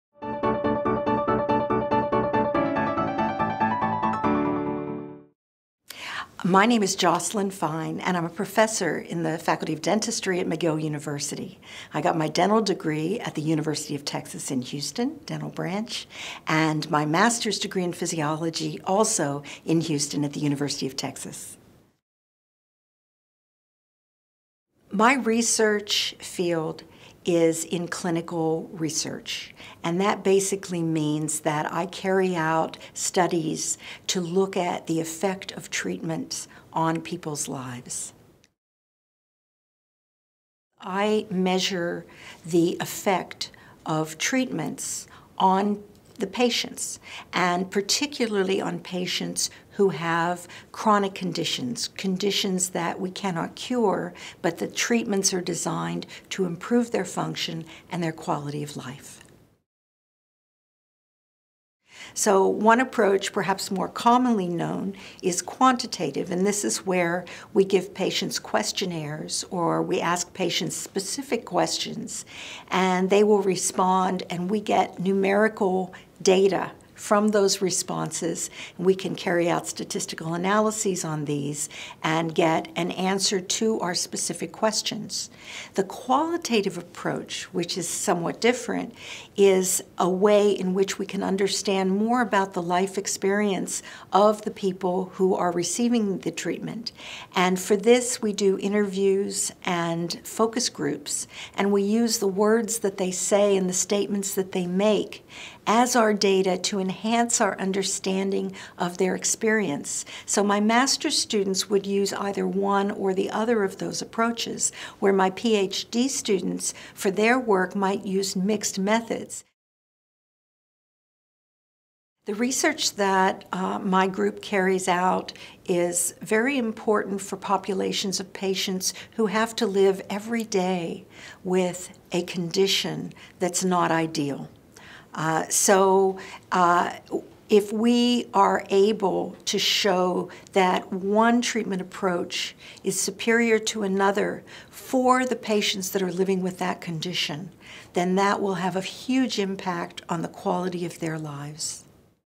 Meet our Professors: Jocelyne Feine
Jocelyne Feine is a clinical researcher. She talks about her research studying chronic disease and finding effective treatments for individuals.